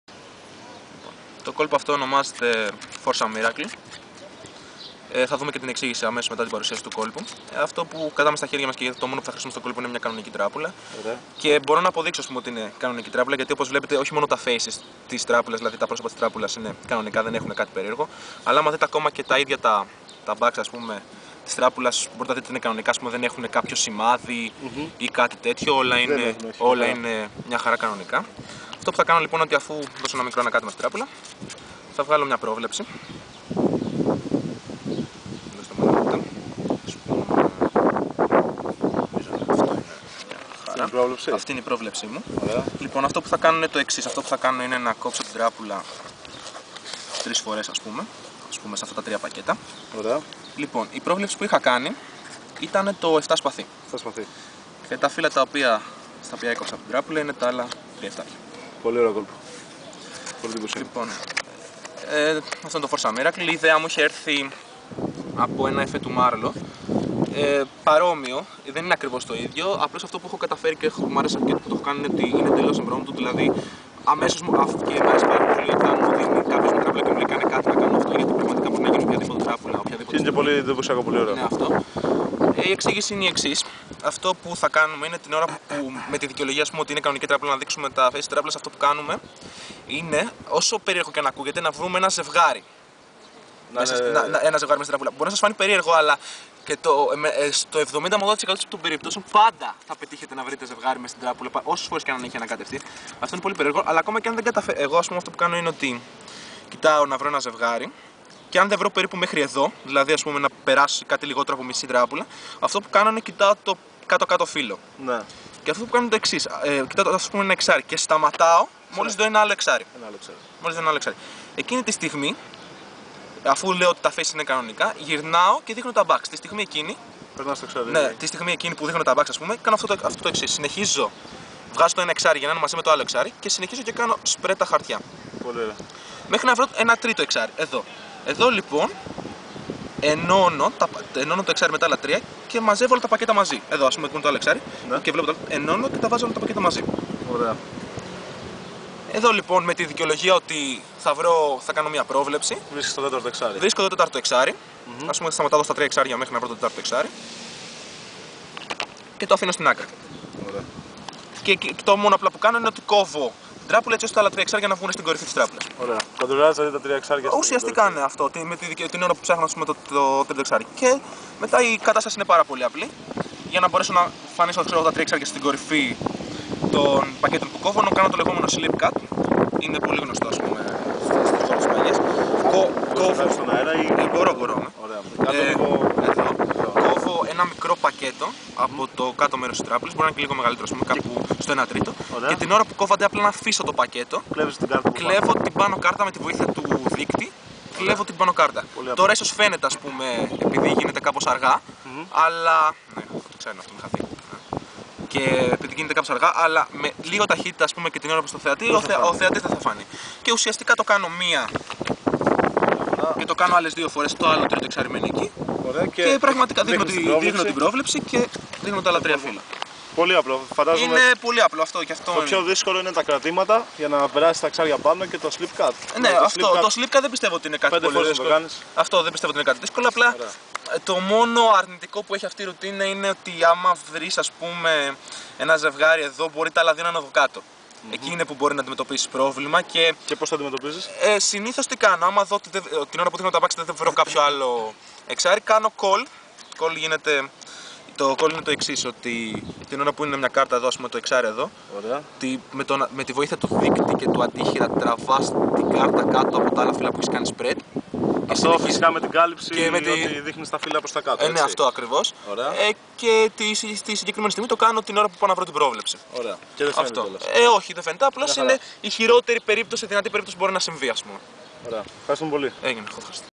U.D. Card Tricks - Foursome miracle (Performed and explained by Bill Botonis)
Here we are again! After a short absence of two weeks we finally upload our new video, in which you can watch Bill's own creation, Foursome miracle! A really spectacular trick!